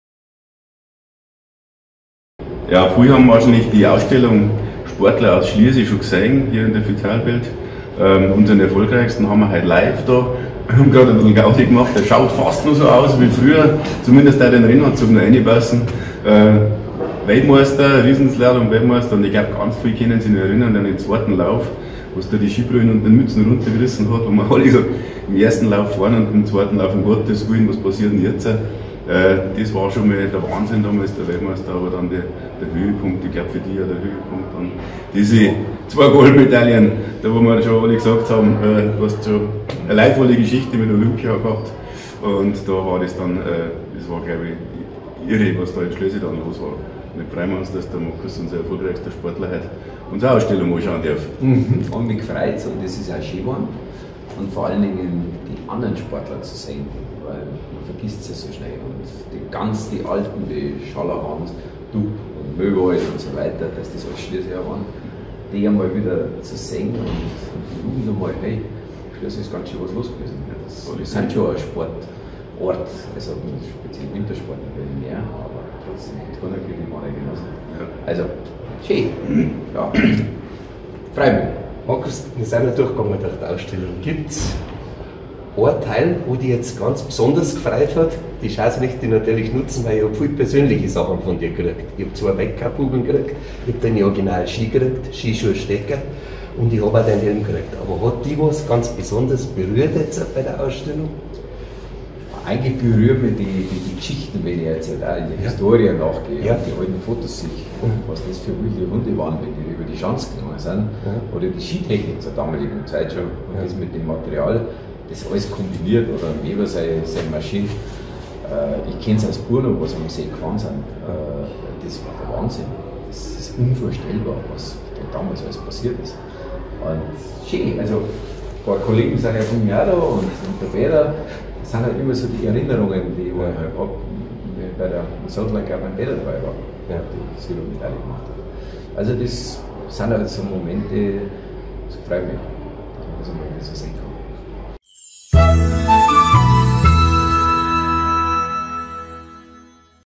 Schliersees erfolgreichster Sportler | Markus Wasmeier
Ausstellung „Sport ohne Grenzen“
Anlässlich des zehnjährigen Jubiläums der Vitalwelt Schliersee findet momentan die Ausstellung „Sport ohne Grenzen“ statt. Der Plakat-Titelheld der Ausstellung ist Schliersees ältester, lebender Weltmeister Hans Schaller, dessen original Rennrodel und Lederhelm sowie seine WM-Medaille in der Ausstellung bewundert werden können. Bundestrainer Peter Schlickenrieder ist mit seinen original Ski, der Silbermedaille sowie Talisman und seinem Rennanzug von Salt Lake City mit dabei. Gezeigt werden über 30 Schlierseer Sportlegenden, und die Facette reicht vom Olympiasieger, Weltmeister, Europameister, Deutscher Meister, Ironman-Teilnehmer zum Bayerischen Meister. Die Konzeption der Ausstellung von Kuramtsleiter Mathias Schrön und seinem Team ist sehr abwechslungsreich und gelungen. Nicht verpassen!
Markus Wasmeier zu Gast in der Ausstellung
Letzte Woche haben Bürgermeister Franz Schnitzenbaumer und Kuramtsleiter Mathias Schrön den Ehrenbürger und erfolgreichsten Sportler von Schliersee, Doppel-Olympiasieger und Weltmeister Markus Wasmeier in der Ausstellung persönlich empfangen. Zusammen haben sie einen Rundgang gemacht, und Markus Wasmeier hat seine Stehle in der Ausstellung signiert. Markus Wasmeier freute sich besonders, die vielen anderen Schlierseer Sportler zu sehen. „In Schliersee ist ganz schön was los, wir sind schon ein Sport-Ort“, lachte er stolz. Wasmeier war beindruckt von den Geschichten, den alten Fotos und freute sich über viele Erinnerungen, die ihm dadurch wieder ins Gedächtnis gerufen wurden. Mathias Schrön fragte Markus Wasmeier nach einem besonderen Moment in seiner Karriere, der ihn am meisten bewegt hat. „Das ist gar nicht so einfach, weil es so viele Momente gibt. Allein bei drei Olympiaden als Favorit an den Start zu gehen und es bei zweien einfach nicht zu schaffen. Egal – einmal einfädeln, einmal Vierter werden. Und dann halt diese Medaillen doch zu bekommen“, sagte Wasmeier. Zu sehen sind seine original Ski, Stöcke, Helm und Brille vom Olympiasieg in Lillehammer sowie beide Weltcup-Kugeln. Markus Wasmeier ist der erfolgreichste Schlierseer Sportler: 1985 wurde er Weltmeister im Riesenslalom in Bormio und 1994 zweifacher Olympia-Sieger im Super-G und Riesenslalom in Lillehammer.
Die Ausstellung „Sport ohne Grenzen“ in der Schlierseer Vitalwelt ist noch bis 15. Januar 2019 zu sehen.
Gäste-Information Schliersee
Perfallstr. 4
83727 Schliersee
Telefon: 08026/6065-0

Öffnungszeiten:
Mo.–Fr. 08:30‒18:00 Uhr
Sa., So., Feiertag 09:00‒13:00 Uhr